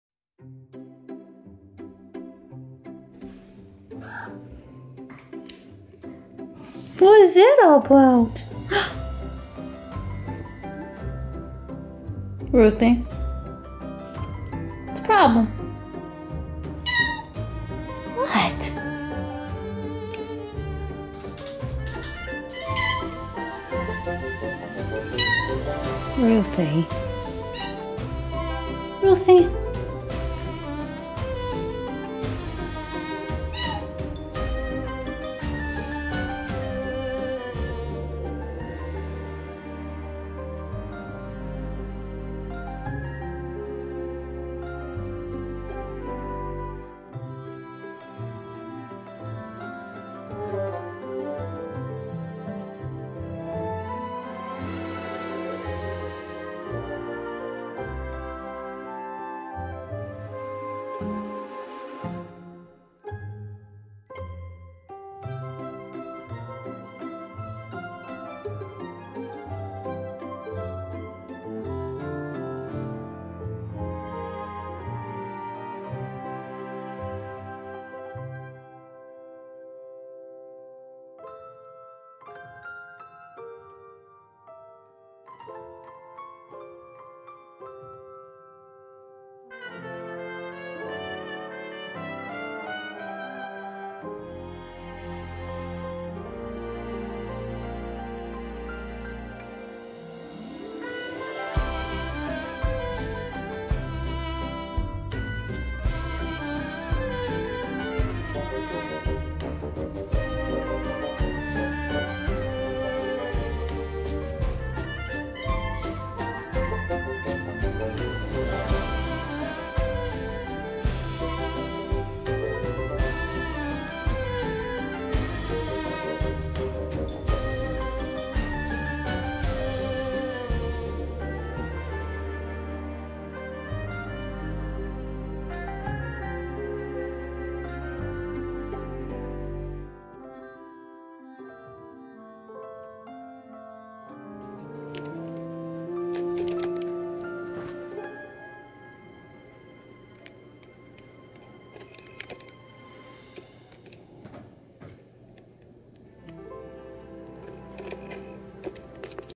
Cat Wants to Play When I'm Trying to Work
My cat doesn't understand the concept of working when there is play to be had.